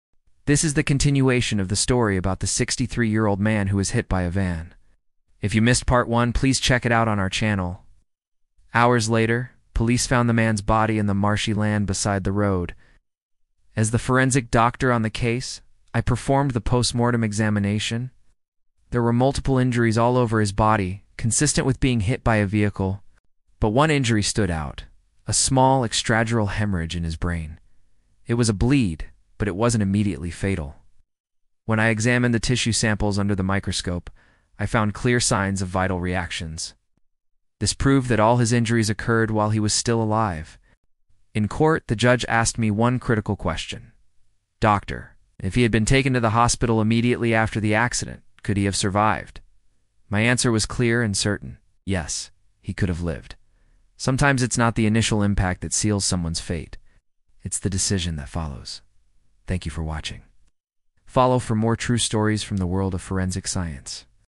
He Could Have Lived – A Forensic Tragedy Part 2
A 63-year-old man was hit by a van on a lonely road.
The driver stopped, looked at him — still alive — and promised to take him to the hospital.
But instead, he drove to a marshy land and left him there to die.

Hours later, the police found the man’s body.
I performed the postmortem examination — and what we discovered revealed the truth.

🧬 His injuries were from a road traffic accident.
🧠 He had a small extradural haemorrhage — not instantly fatal.
🩸 No postmortem injuries. No dragging marks.
📜 Histology confirmed the wounds were antemortem, meaning he was alive after the accident.

The court asked me one question:

“Doctor, could he have survived if he was taken to the hospital?”

My answer was simple —
Yes. He could have lived.

This is a real forensic case , showing how neglect can turn an accident into a tragedy.